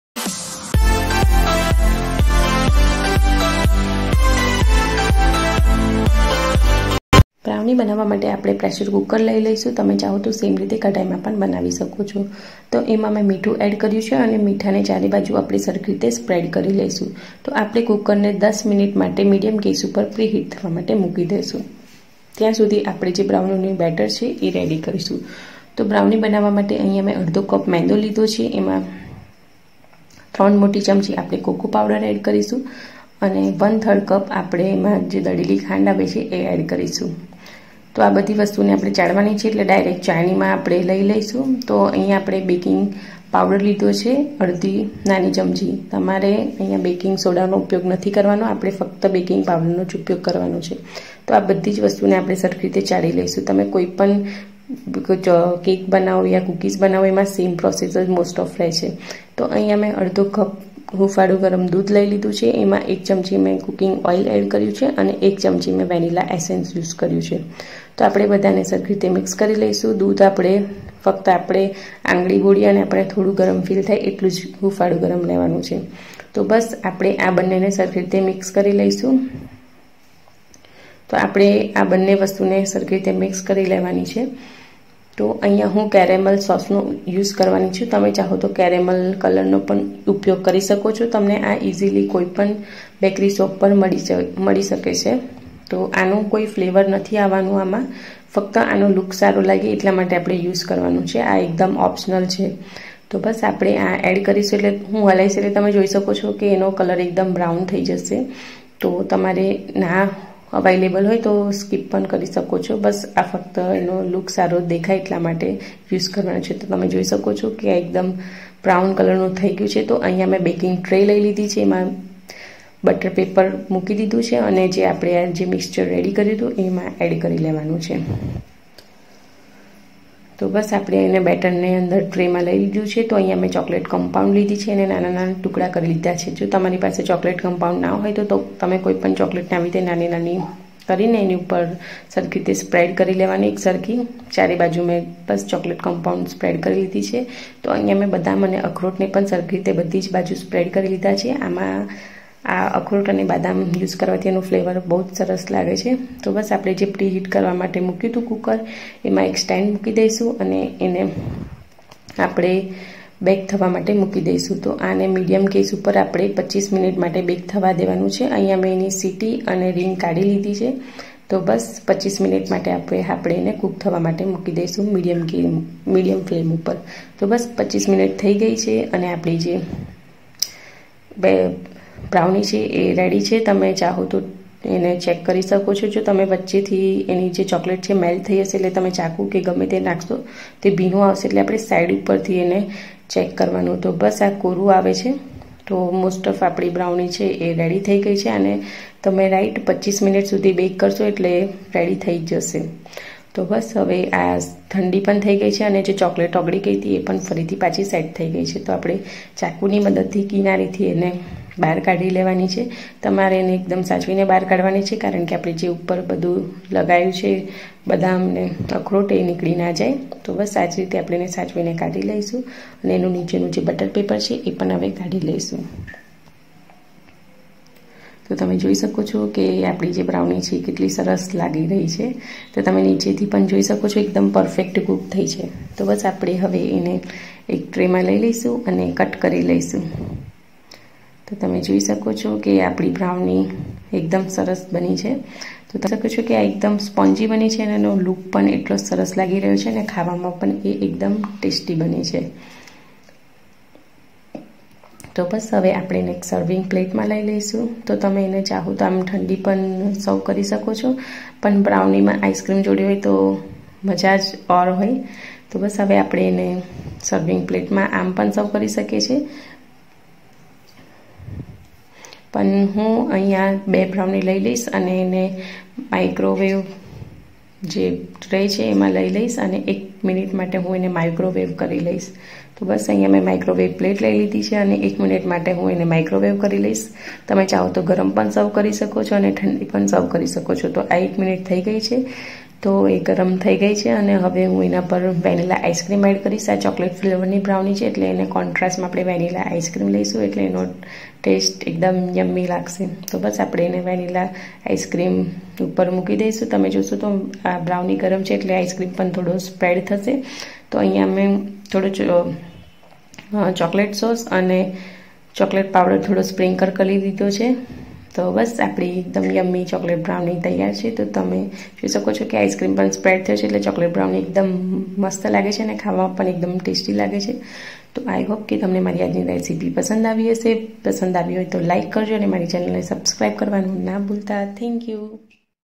કુકરમાં બ્રાઉની ઈંડા કન્ડેન્સ મિલ્ક બટર વગર |એગલેસ ચોકલેટ બ્રાઉની
તૈયાર થવા માટે લાગતો સમય-૪૦મિનીટ
serving--4
મેંદો -૧/૨ કપ
દળેલી ખાંડ-૧/૩ કપ
કોકો પાવડર -૩ tbsp
બેકિંગ પાઉડર_1/2 tsp
દૂધ-1/2 કપ
તેલ -૧ tbsp
વેનિલા એસેન્સ -1tsp
કેરેમલ કલર - 1/2tsp
 For garnishing -ડાર્ક ચોકલેટ બદામઅખરોટ